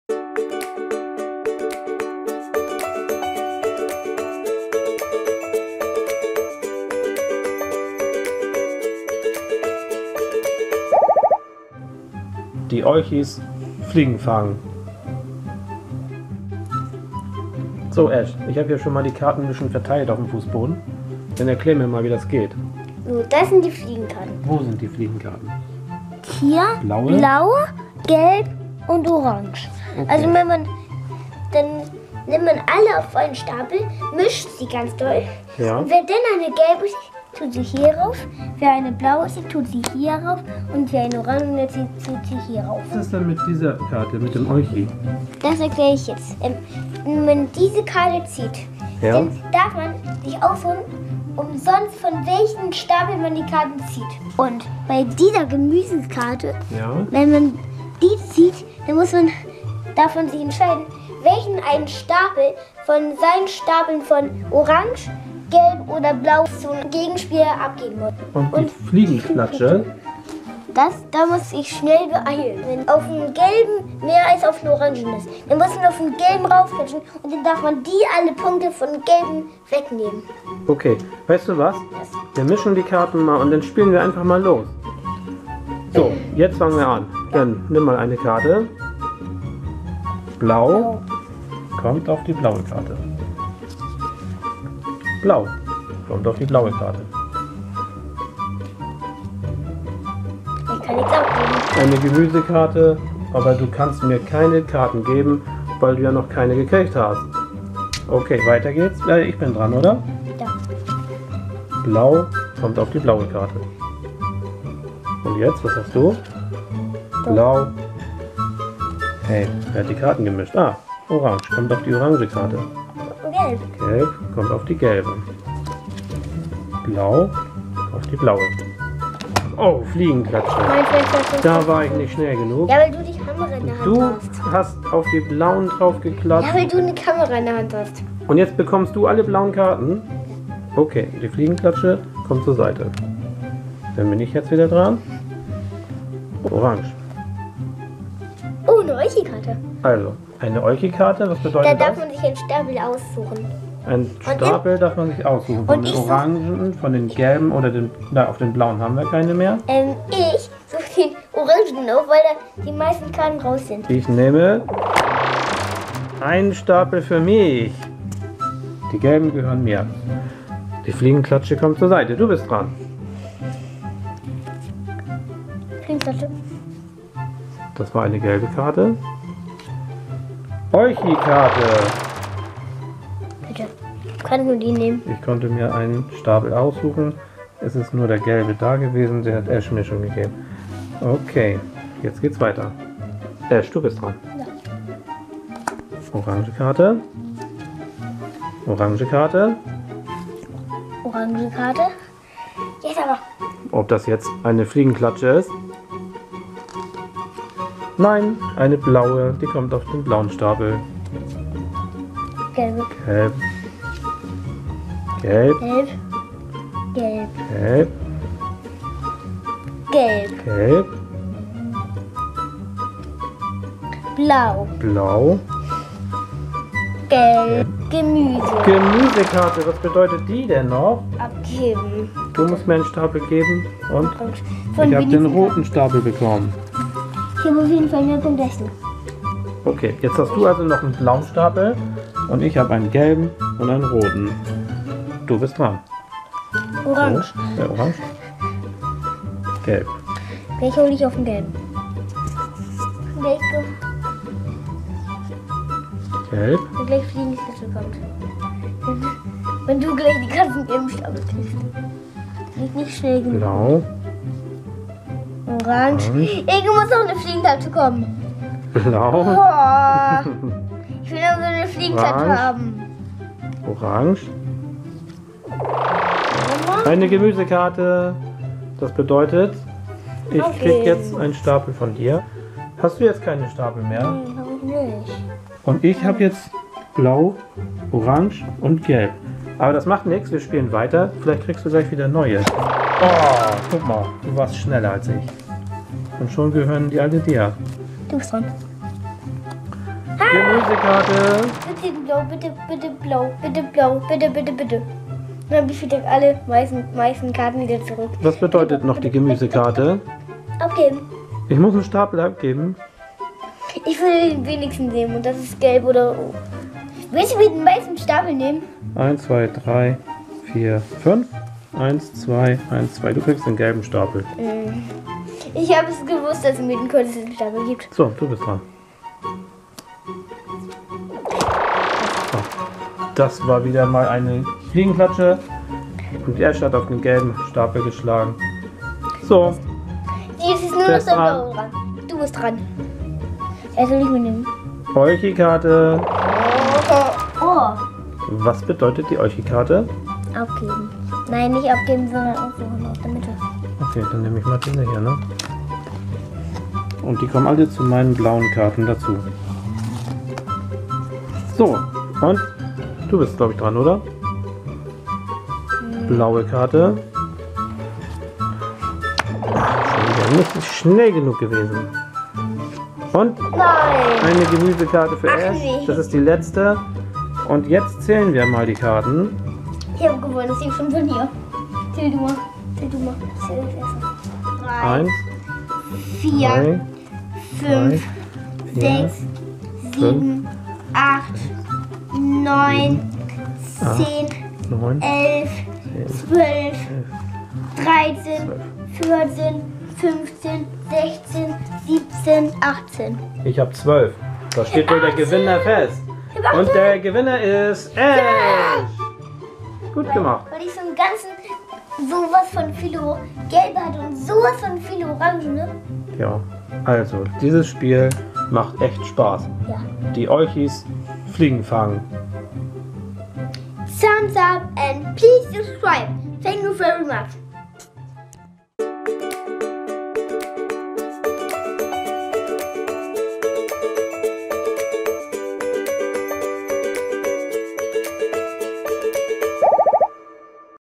Die Olchis - Wir spielen "Fliegen Fangen" Spielzeug für Ash/ Kanal für Kinder Kinderkanal
TipTapTube - Kanal für Kinder - Kinderkanal
Wir spielen " Die Olchis - Fliegen fangen" ein Spielzeug für flinke Finger ab 4 Jahren für 2-6 Spieler:
Wo olchiger Stinkermüll ist, sind Fliegen nicht weit! In diesem Spiel sind wir die Olchies und müssen so viele Fliegen wie möglich fangen. Denn wer am Ende die meisten Fliegen gefangen hat, ist der oberstinkigste Olchi! Doch aufgepasst - in den Tiefen des Abfalls lauern oft gesunde Vitamine!